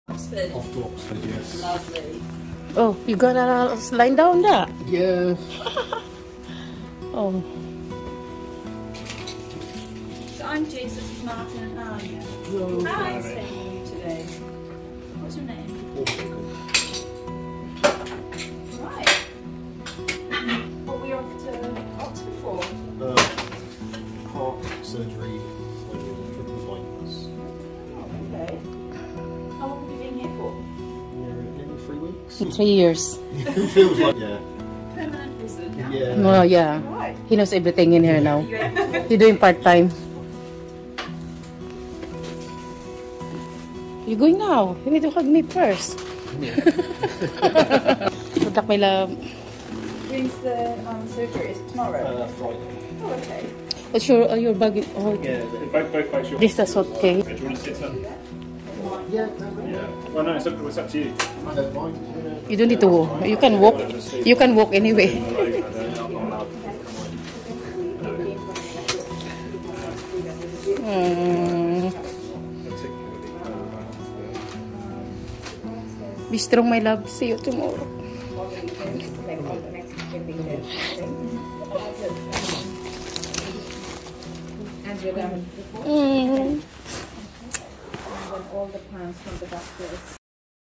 Ambulance Arrived || Coneyslifestylein uk
Ambulance Arrived Ready to Transfer him to Hospital for his heart operation be Strong sweetheart ❤️  || Coneyslifestylein uk